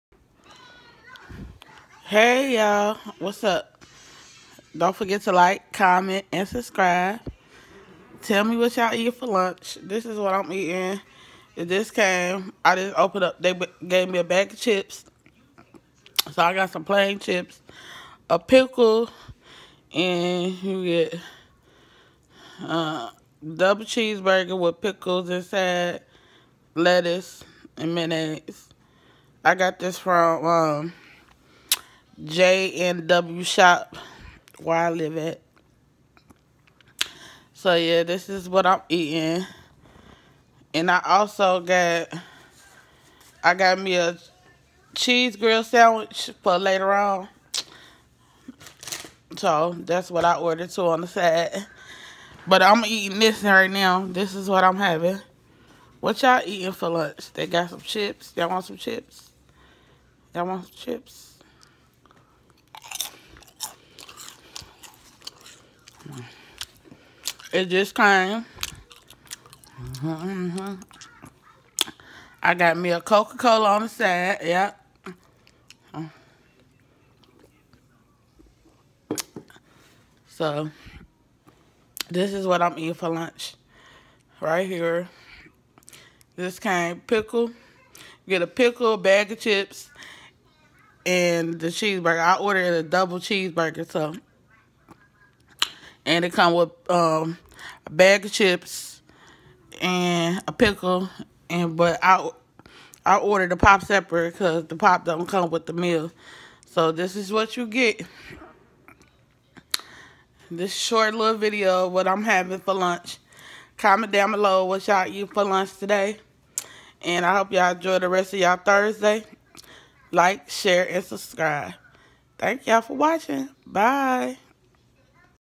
October 8, 2020
What’s for lunch today me a double cheese burger 🍔 chips and pickle n coke